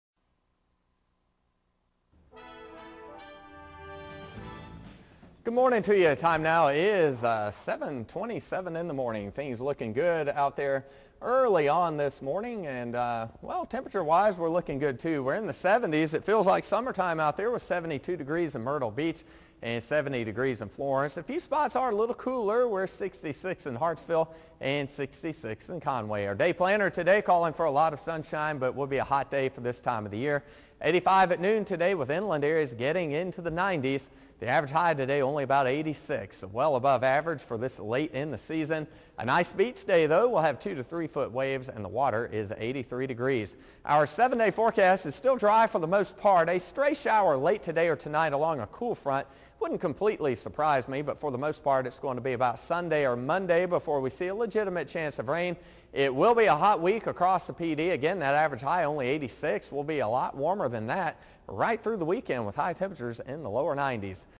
Wednesday morning weather forecast for Sept. 8, 2010
Your weather forecast for the Pee Dee and Grand Strand communities of northeastern South Carolina.